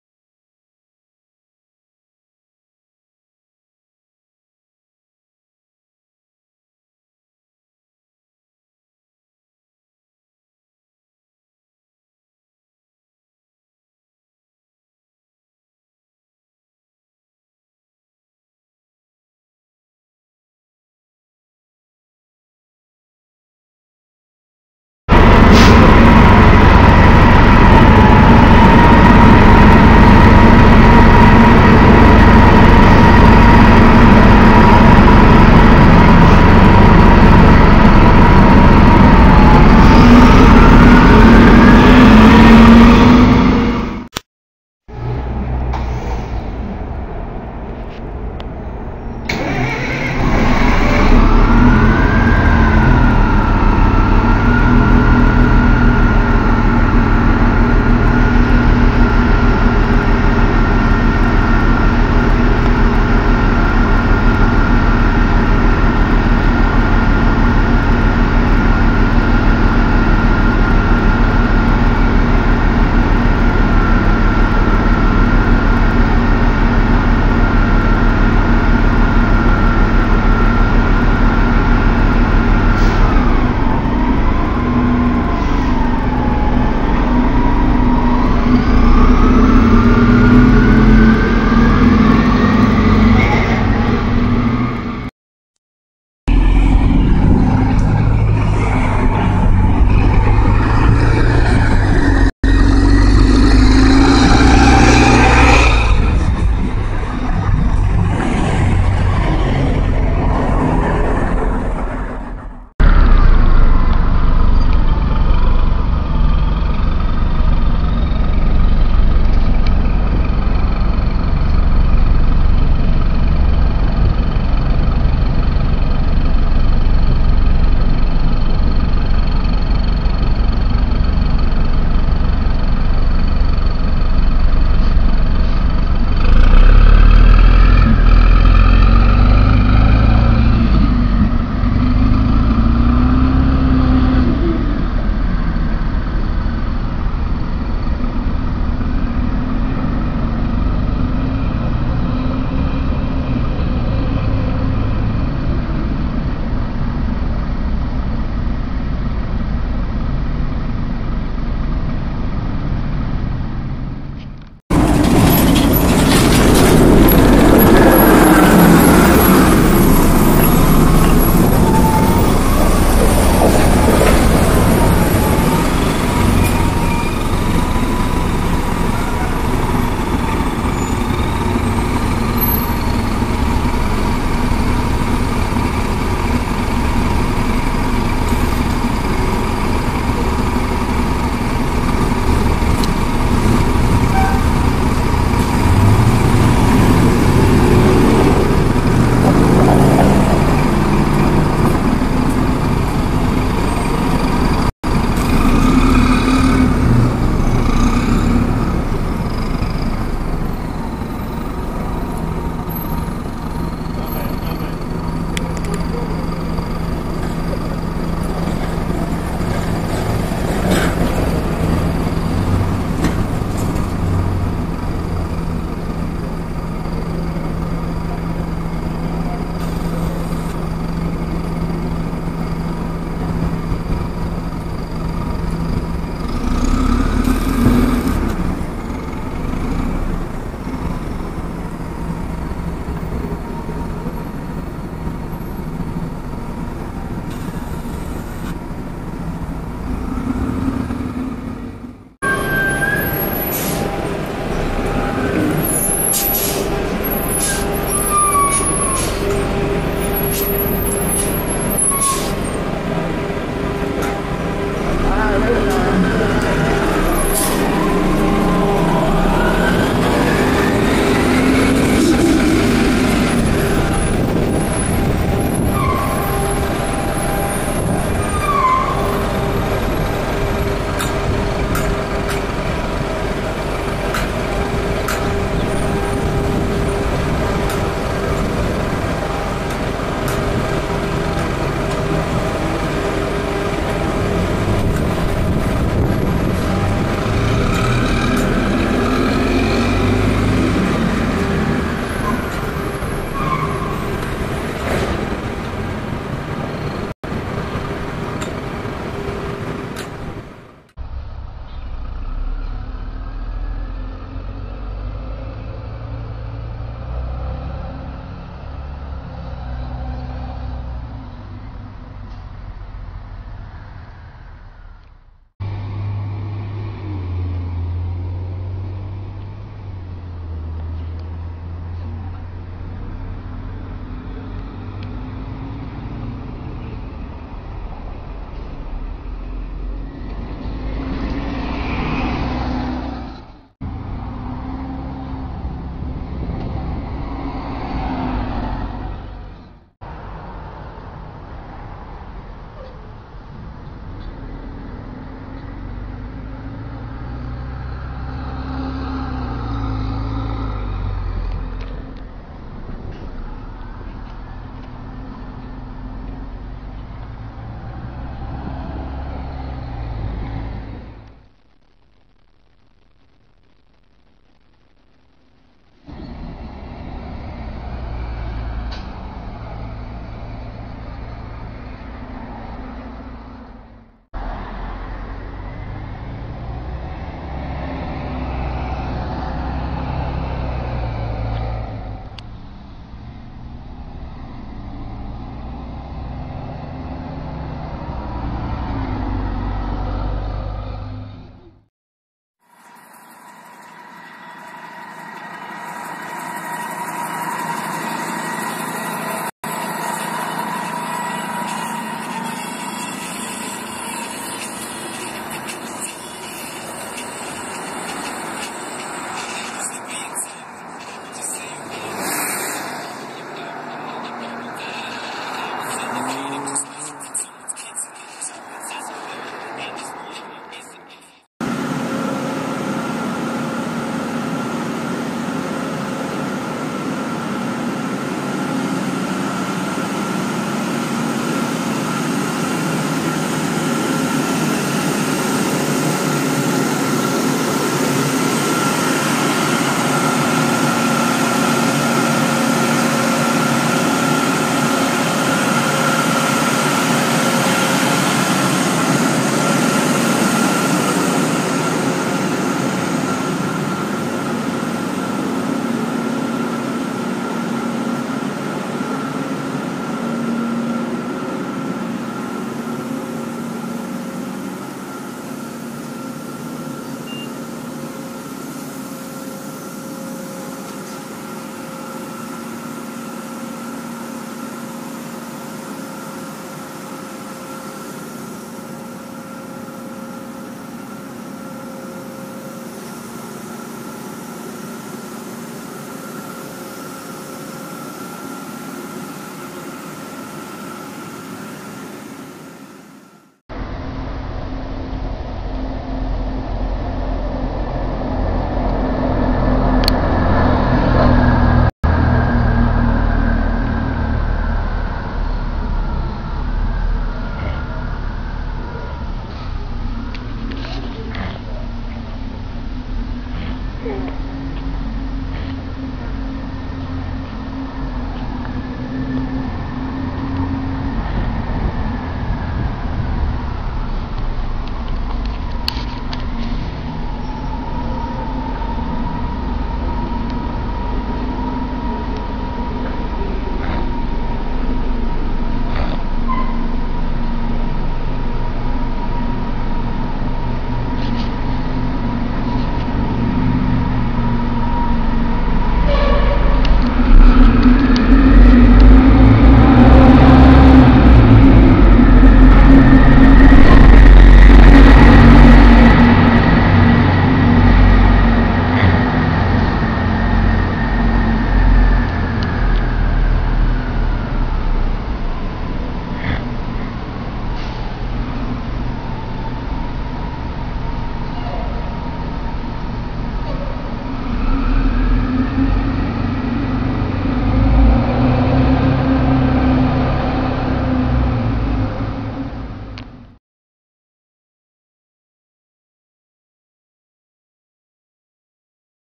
Marta Bus: 2002 Orion VII CNG 2101-2160. Greatest Moments! Compilation!
A Tribute to orion Cng buses been sadly gone since 2015.